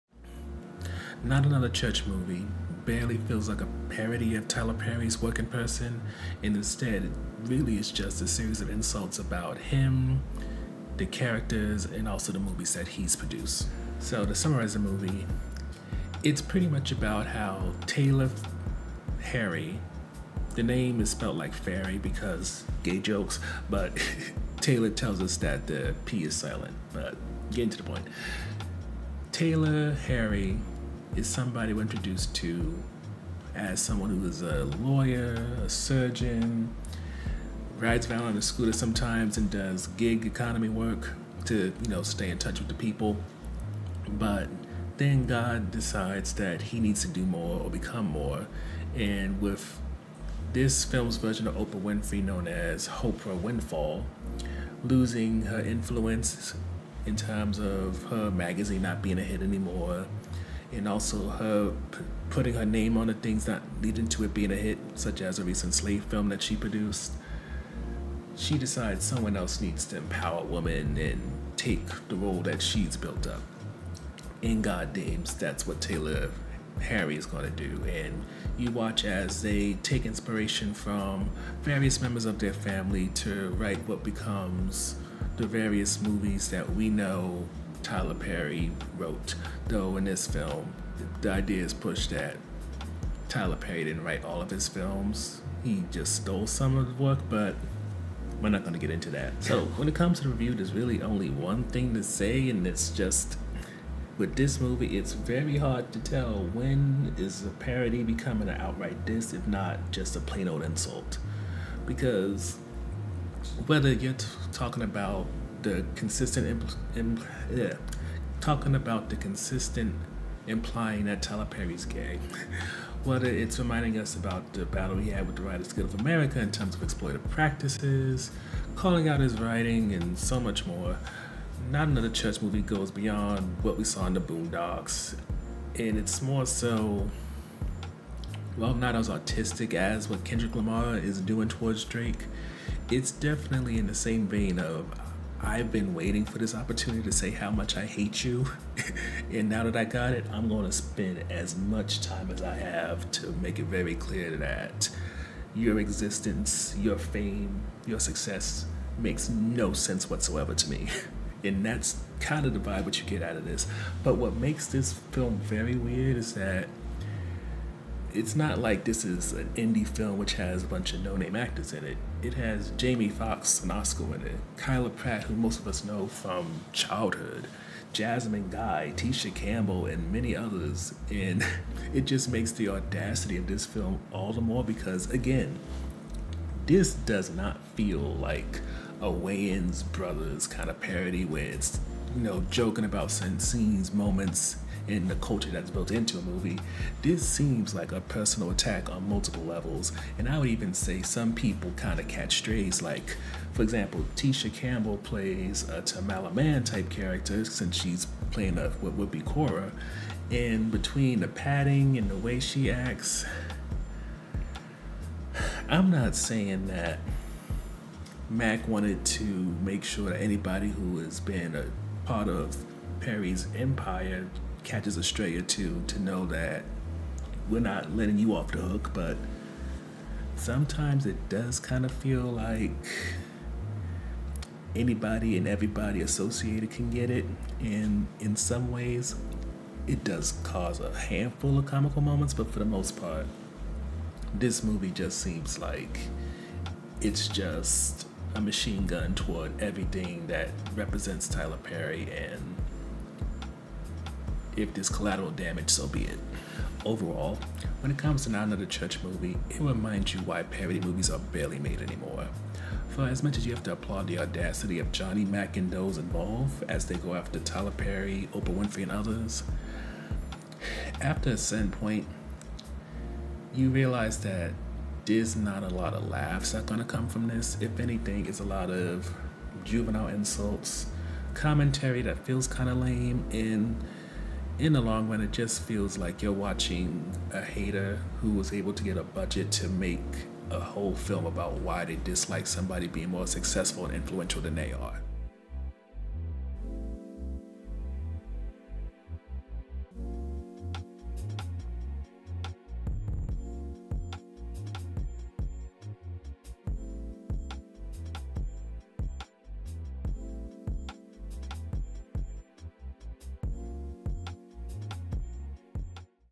Not Another Church Movie (2024) - Video Review
“Not Another church Movie” is barely a parody of Tyler Perry’s work and person, it is an hour-and-a-half series of insults.

Beat Produced by Mad Keys

0:00 Overview
0:13 Summary
1:39 Review